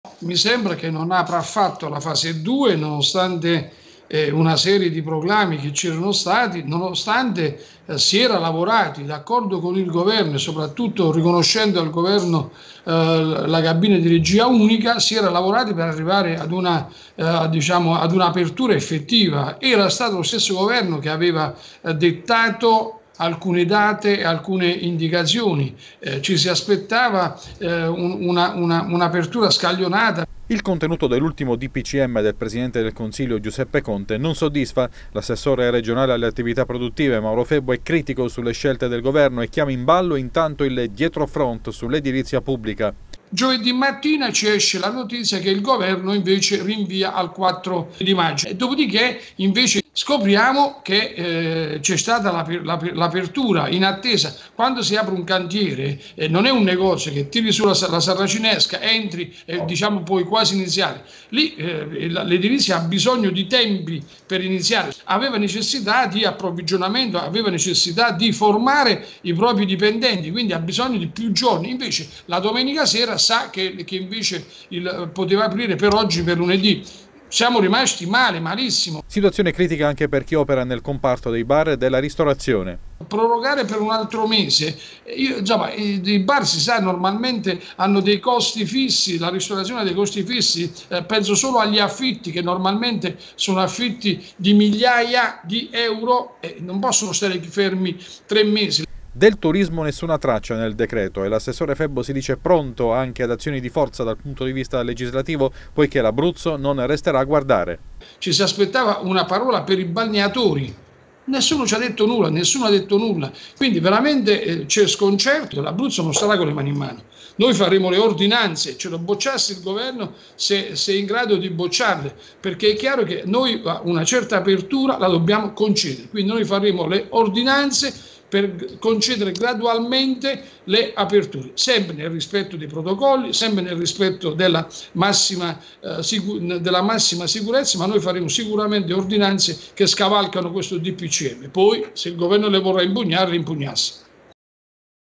Febbo: “Il Dpcm non apre la fase 2”
L’ultimo decreto del premier Conte non apre la fase 2 e non è in linea con le indicazioni fornite nella cabina di regia coordinata dal Governo. E’ questa la critica rivolta dall’assessore regionale Mauro Febbo ai provvedimenti presi con l’ultimo Dpcm